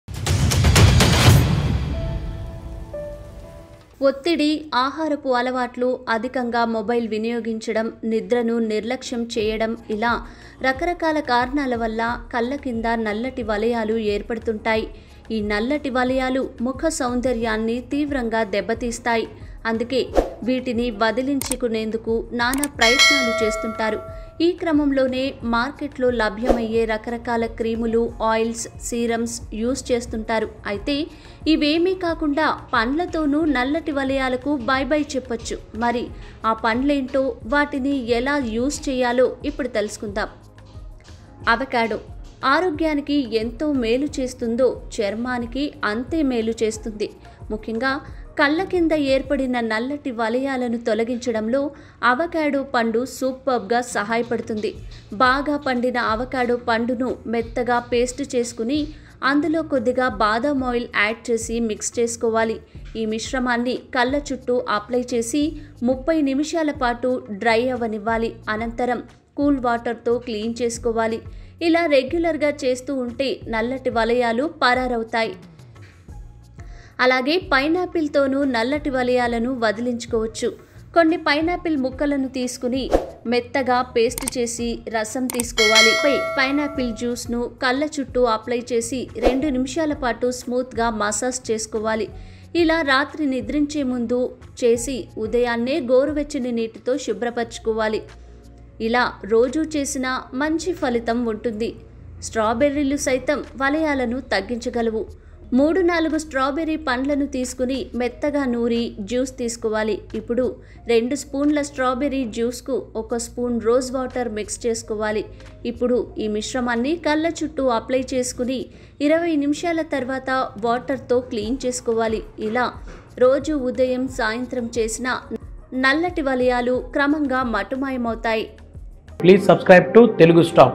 Fruits that help in reducing Under Eye Dark Circles| కళ్ళ కింద ఉన్న నల్లటి వలయాలు పోవాలంటే
dark circles under the eyes can indicate a need for lifestyle changes, such as improvements to sleep habits or diet.The area under the eyes can appear darker due to constricted blood vessels causing hyperpigmentation, or from thinning of the skin around the eyes.The skin around our eyes are the most sensitive and delicate skin of the entire face. Fruits that helps in reducing and preventing under eye circles
 Avocado For Under Eye Dark Circles:
Avocado Paste With Badam Oil.
Pine Apple :
Apply Pine Apple Juice Around Eyes.
Straw Berries :
Straw berry Juice with Rose Water.